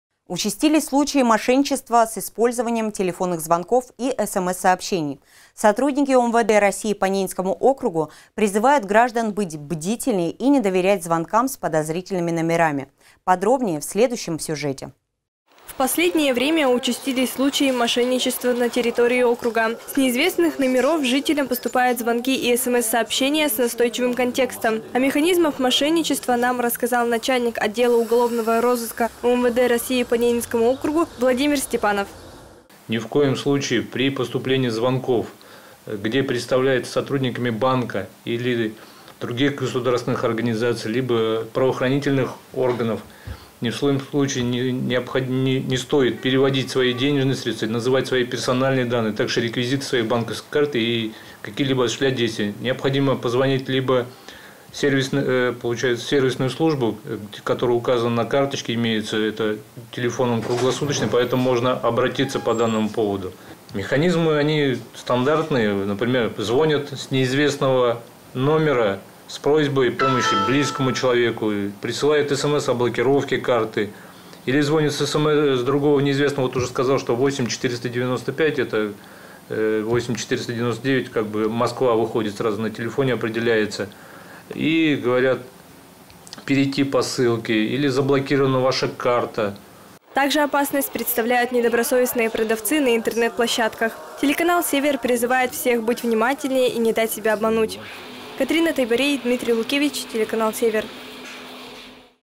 МОШЕННИКИ В ДЕЛЕ
Участились случаи мошенничества с использованием телефонных звонков и смс сообщений. Сотрудники УМВД России по Ненецкому округу призывают граждан быть бдительнее и не доверять звонкам с подозрительными номерами.